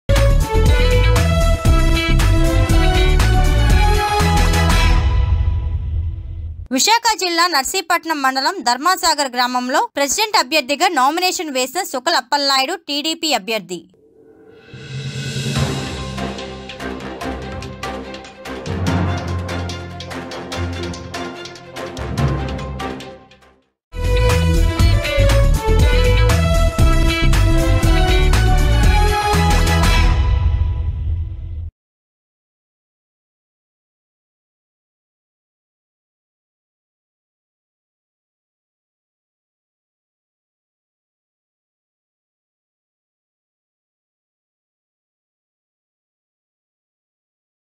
విశాఖ నర్సీపట్నం ధర్మసాగర్ లో ప్రెసిడెంట్ అభ్యర్థిగా నామినేషన్ వేసిన సుకల అప్పలనాయుడు...
@truthnewstelugu Sukala Appalanaidu has been nominated as the presidential candidate in Visakhapatnam Narsipatnam Dharmasagar.